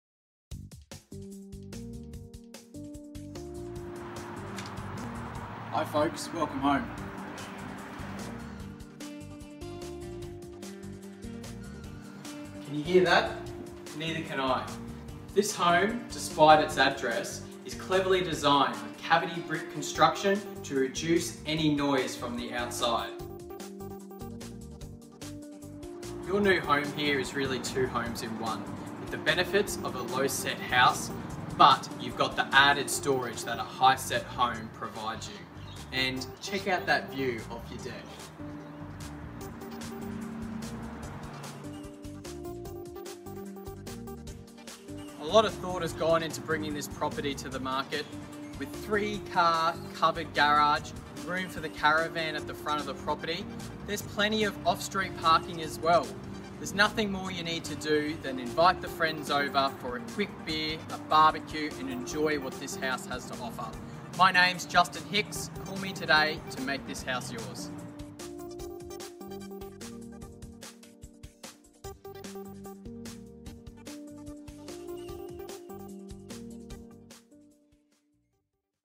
87 Old Northern Road, Everton Park Madeleine Hicks Real Estate Everton Park, Brisbane
87 Old Northern Road, Everton Park was designed for you.

This home has all the comforts of a low set home but the conveniences of storage and garage space of a two-story home. Sitting High on the hill overlooking the prestigious Trouts estate your new home will be the talk around town.

Not only does it have 3 car garages with plenty of storage for all your toys, this home has been designed to suit everyone. Room for the caravan, and boat at the front of the property as well. If you're downsizing you will love that all the living areas are on the on level. No need for stairs everything you need is right there.

The owner of this wonderful home has put blood, sweat, and tears to make this property perfect for its new owners. Not only will you be moving into the best Suburb in Brisbane but you will be able to move in and do nothing except add your personal touches.